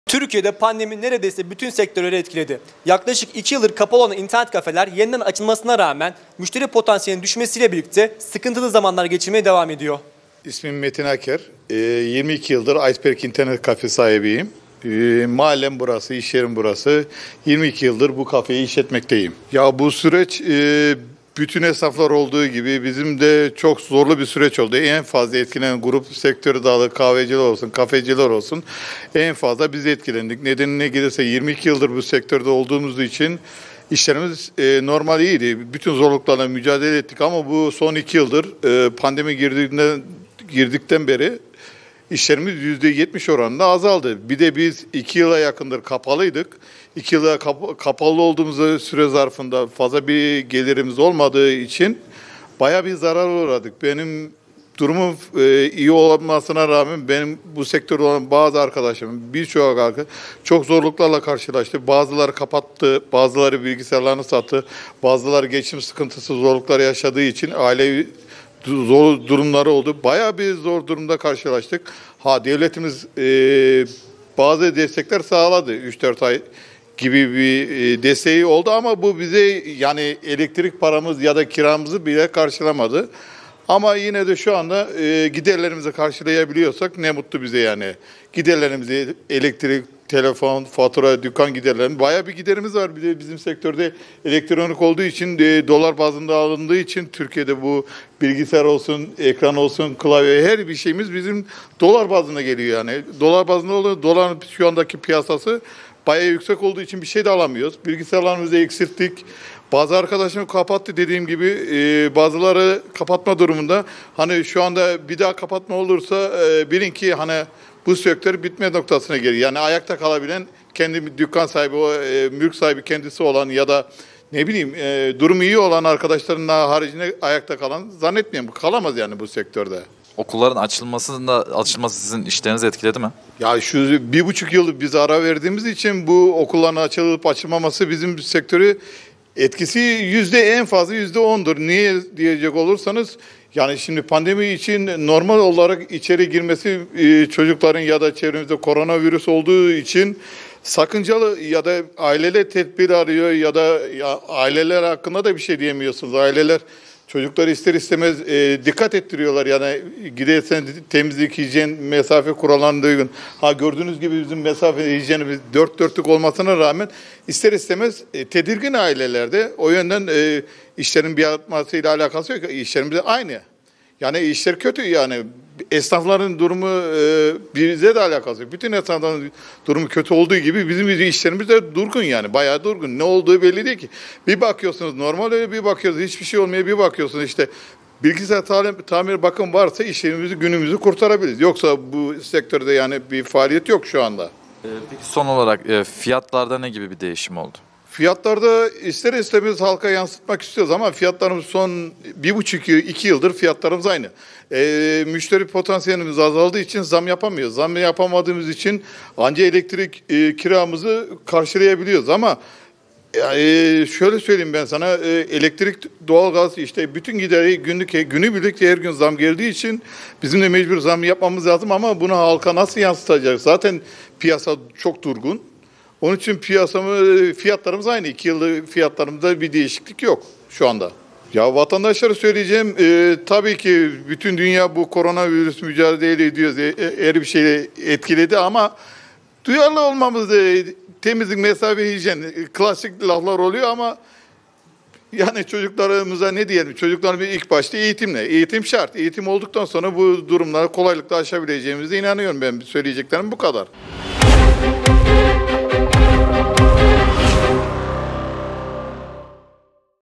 İnternet Kafelere Pandemi Etkisi
KAYSERİ,(BGC) – Dünya genelinde oluşan koronavirüs salgını tüm sektörleri olduğu gibi internet kafe sektörünü de olumsuz yönde etkiledi. İnternet kafe sahibi Metin Aker, “ Son 2 yıldır pandemi girdiğinden beri işlerimiz yüzde 70 oranında azaldı” dedi.

Pandemiyle beraber yaklaşık 2 yıldır bütün sektörlerde olduğu gibi internet kafe sektörü de en çok etkilenen meslekler arasında yer alıyor. 22 yıldır internet kafe işleten Metin Aker yaşadıkları son durumla ilgili açıklamalarda bulundu. Yaşadıkları süreci değerlendiren Aker, “Bu süreç bütün esnaflara olduğu gibi bizim içinde zorlu bir süreç oldu. En çok etkilenen grup olarak en kahveciler olsun ya da kafeciler olsun internet kafeciler olsun en çok biz etkilendik. 22 yıldır bu sektörde olduğumuz için işlerimizi iyiydi ama bunun yanında bütün zorluklarla mücadele ettik. Son 2 yıldır pandemi girdiğinden beri işlerimiz yüzde 70 oranında azaldı. Bir de biz 2 yıla yakındır kapalıydık. Kapalı olduğumuz süre zarfında fazla bir gelirimiz olmadığı için baya bir zarara uğradık. Benim durumum iyi olmasına rağmen, bu sektörde olup zorlanan birçok arkadaşım oldu. Bazıları iş yerlerini kapattı. Bazıları bilgisayarlarını sattı. Bazıları da geçim sıkıntısı ve zorluklar yaşadığı için ailevi zor durumları oldu” diye konuştu.

“BİLGİSAYARLARIMIZI EKSİLTTİK”

Sektör olarak zor durumda olduklarını belirten Aker, “Birçok durumla karşılaştık. Devletimiz yaklaşık 4 ay bazı destekler sağladı. Ama bu destekler bizim elektrik ve kira paramızı karşılamadı. Ama şu anda giderlerimizi karşılayabiliyorsak ne mutlu bize. Epey giderimiz var. Biz elektronik sektöründe olduğumuz için aldığımız bütün ürünler dolar bazlı olduğu için şu anda yeni ürünler alamıyoruz. Bilgisayarlarımızı eksilttik. Eğer ki bir daha bir kapatma olursa bilin ki bu sektör bitme noktasına gelir. Ayakta kalabilen ya da kendi mülkünde bu işi yapıp durumu iyi olan arkadaşlar haricinde ayakta kalabilen olduğunu sanmıyorum” ifadelerini kullandı.